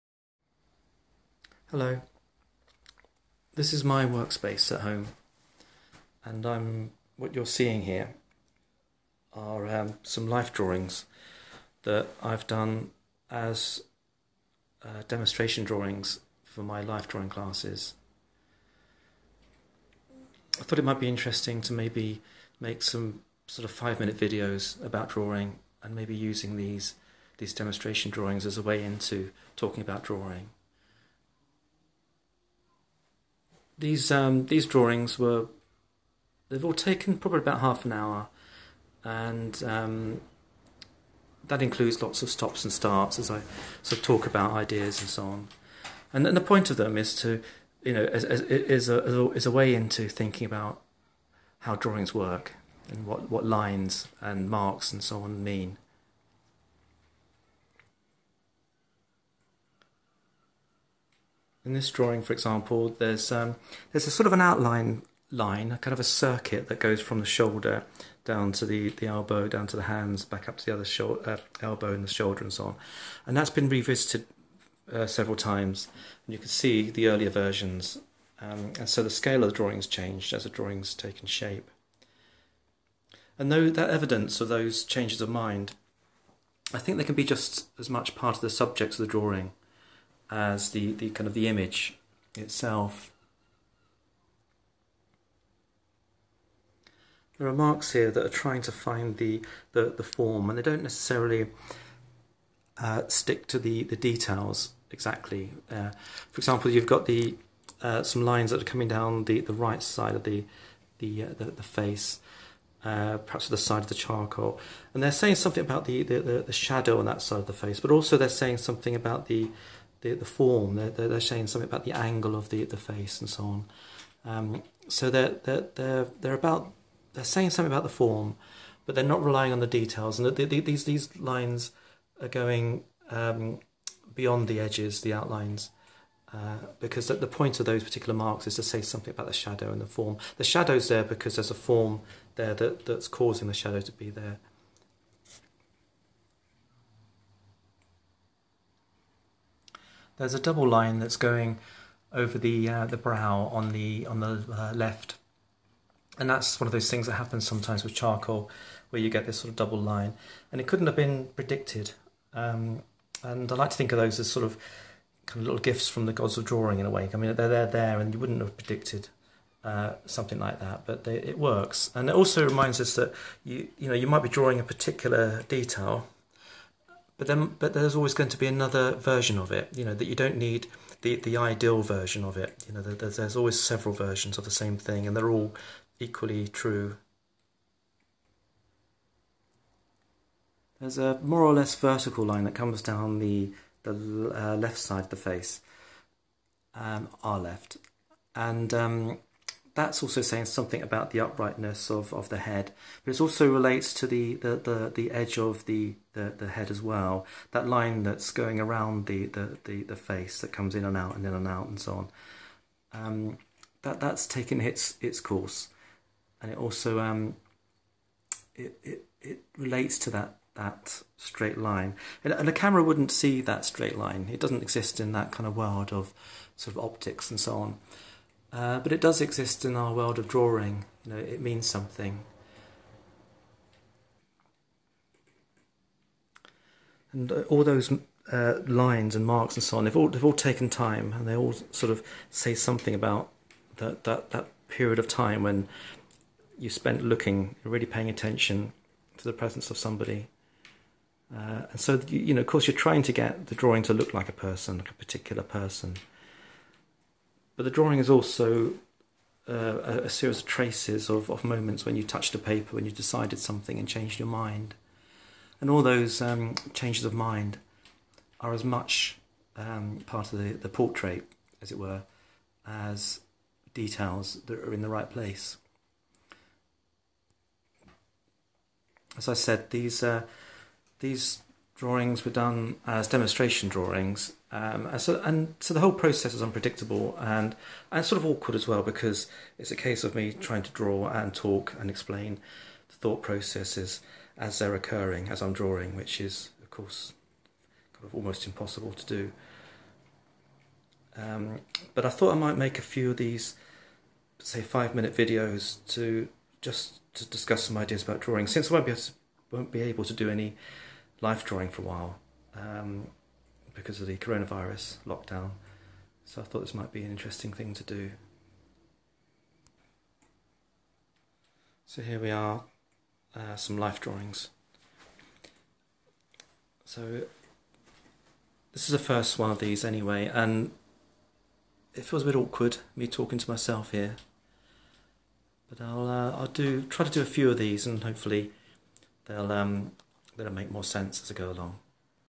Life Drawing
Some thoughts about Drawing, using my demonstration drawings from my Life Drawing classes at Margate Adult Education Centre in Hawley Square.

Since Life Drawing isn't possible for as long as the Coronavirus lockdown continues, i thought it might be interesting to do a series of short videos about Drawing using some of my demonstration drawings. This is my first attempt. In this video I say a few things that come to mind as we look at some drawings. My plan to make a number if five-minute videos and to focus on a different drawing for each video. This idea might evolve and change as I do more of them.

If you're interested in my ideas about Drawing, you can find more on my wordpress blog: "Roy Eastland Drawing".